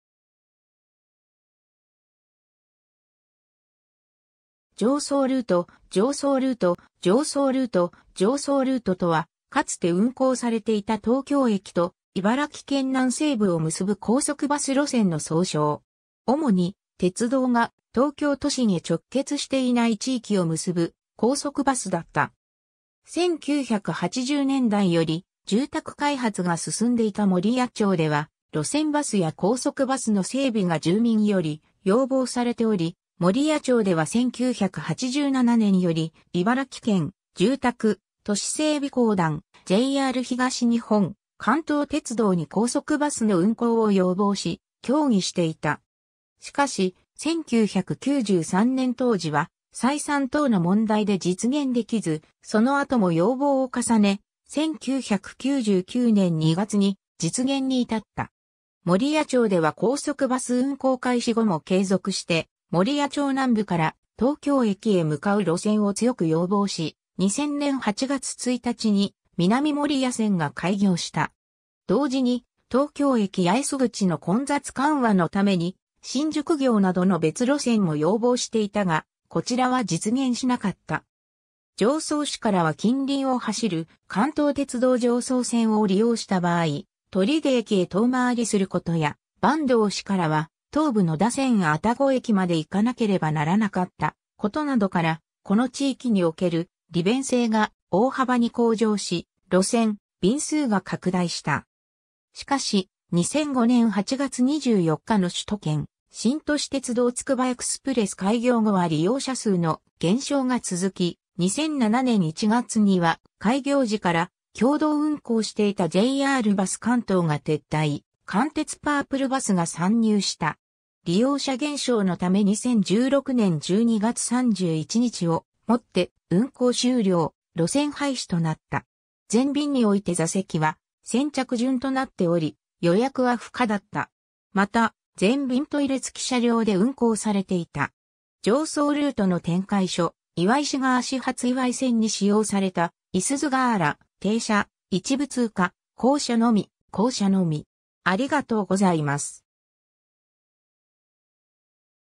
常総ルート (高速バス)
高速バス路線_(廃止)
常総ルート（関東鉄道） 常総ルート（関鉄パープルバス） 常総ルート（JRバス関東） 常総ルート（じょうそうルート）とは、かつて運行されていた東京駅と茨城県南西部を結ぶ高速バス路線の総称。
 主に、鉄道が東京都心へ直結していない地域を結ぶ高速バスだった。
1980年代より住宅開発が進んでいた守谷町（現:守谷市）では、路線バスや高速バスの整備が住民より要望されており、守谷町では1987年（昭和62年）より、茨城県、住宅・都市整備公団（現:都市再生機構）、JR東日本、関東鉄道に高速バスの運行を要望し、協議していた。
しかし、1993年（平成5年）当時は採算等の問題で実現できず、その後も要望を重ね、1999年（平成11年）2月に実現に至った。
守谷町では高速バス運行開始後も継続して、守谷町南部から東京駅へ向かう路線を強く要望し、2000年（平成12年）8月1日に南守谷線が開業した。
同時に東京駅八重洲口の混雑緩和のために、新宿行などの別路線も要望していたが、こちらは実現しなかった。
 常総市からは近隣を走る関東鉄道常総線を利用した場合、取手駅へ遠回りすることや、坂東市（岩井地区）からは東武野田線愛宕駅まで行かなければならなかったことなどから、この地域における利便性が大幅に向上し、路線・便数が拡大した。
しかし、2005年（平成17年）8月24日の首都圏新都市鉄道つくばエクスプレス開業後は利用者数の減少が続き、2007年（平成19年）1月には開業時から共同運行していたジェイアールバス関東が撤退、関鉄パープルバスが参入した。
 利用者減少のため2016年（平成28年）12月31日をもって運行終了、路線廃止となった。
 全便において座席は先着順となっており、予約は不可だった。
また、全便トイレつき車両で運行されていた。
 常総ルート（岩井線）の転回所・岩井市側始発 岩井線に使用された，いすゞ・ガーラ ●:停車（終日上下便）■:一部通過（平日・土曜上下便） ○:降車のみ（平日・土曜上り便） △:降車のみ（終日上り便）